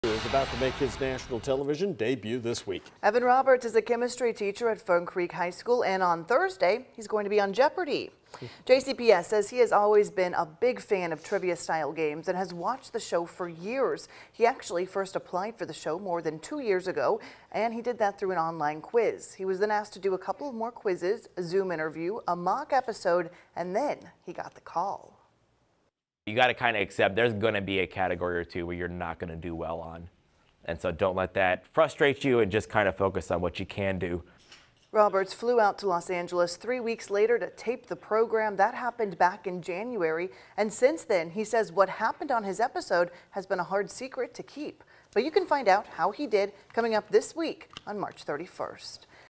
Fern Creek High School chemistry teacher appears on Jeopardy!
Jefferson County Public Schools said he has always been a fan of trivia-style games and has watched the show for years.